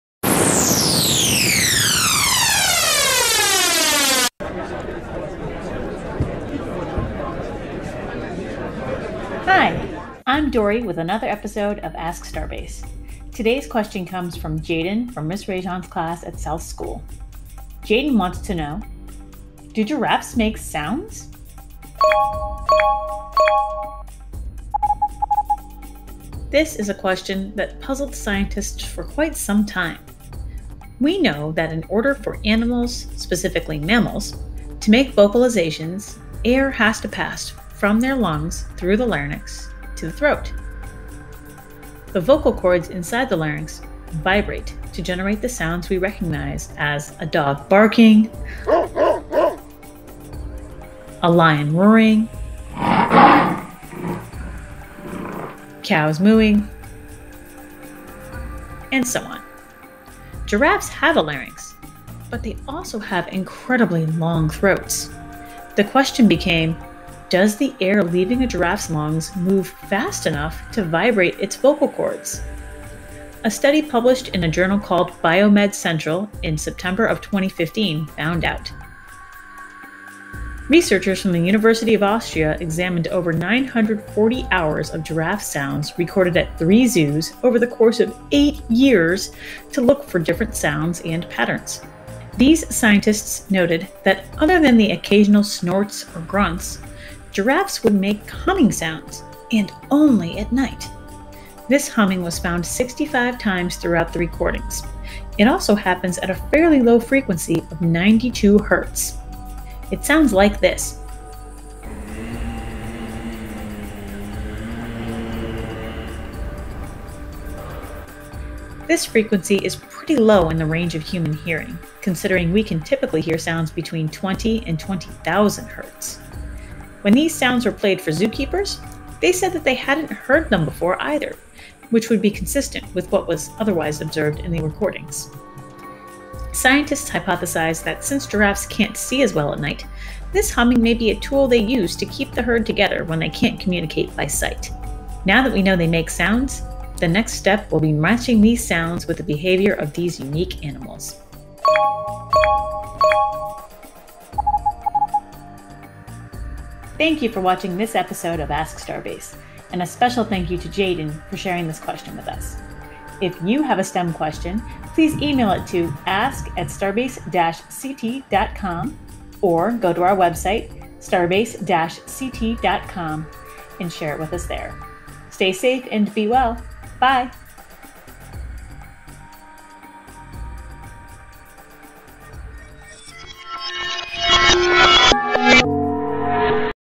Ask STARBASE Episode 091: Do giraffes make sounds?
ASK STARBASE Episode 091:Do giraffes make sounds? Show Notes